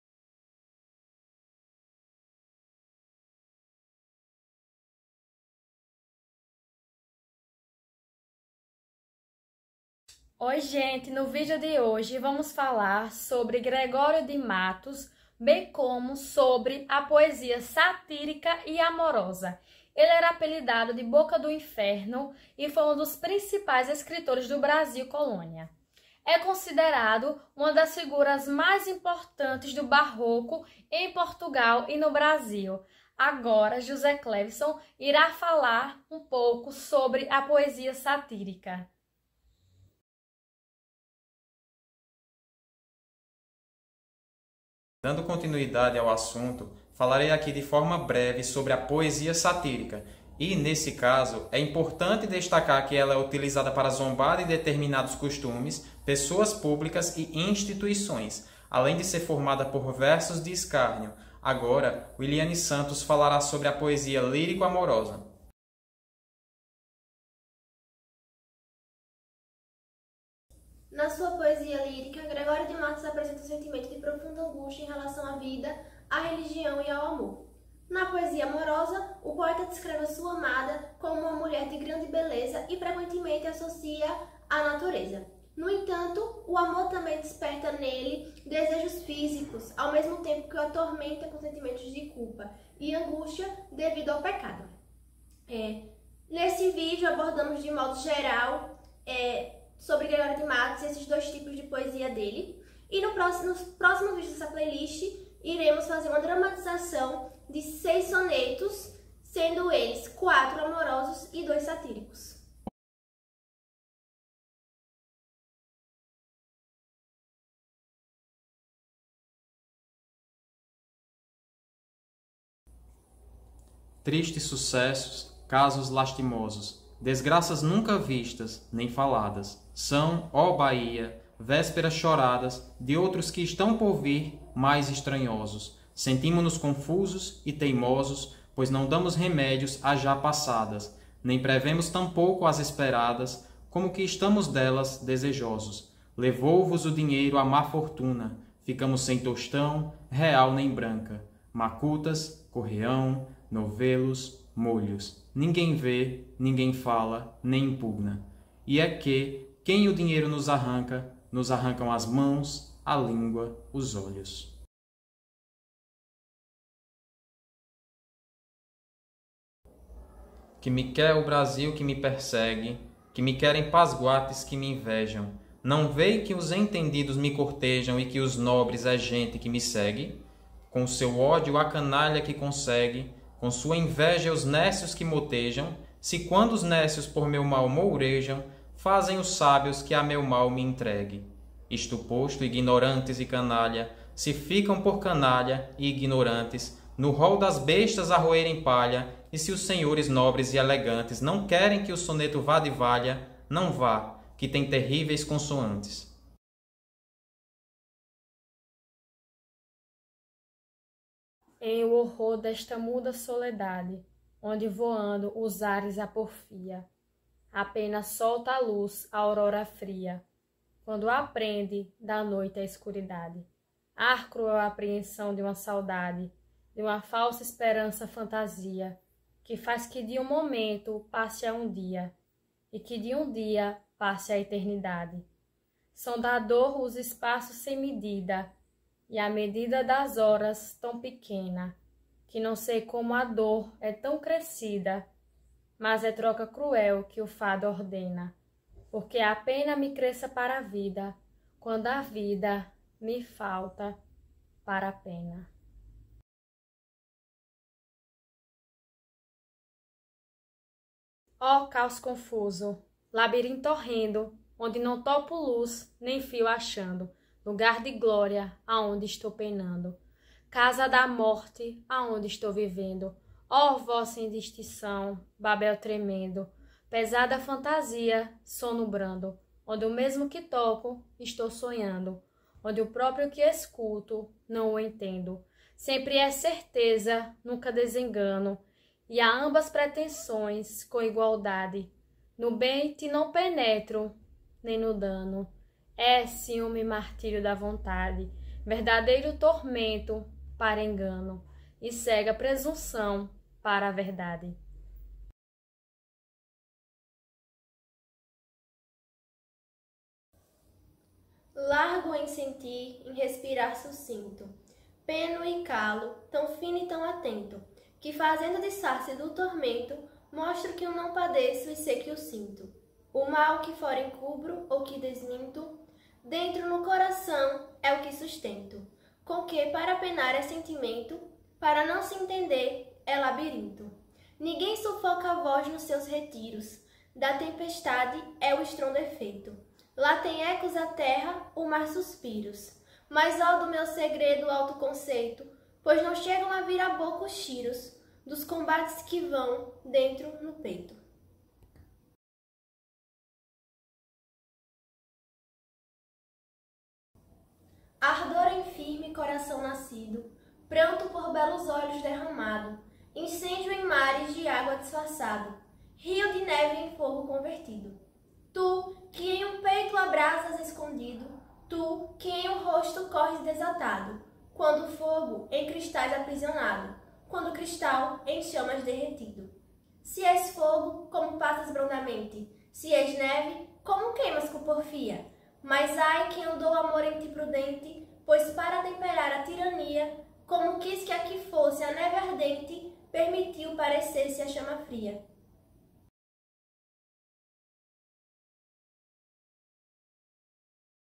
Leitura compartilhada _ Gregório de Matos Guerra
Neste vídeo, o grupo Apoena apresenta poemas satíricos e lírico-amorosos do poeta baiano Gregório de Matos Guerra.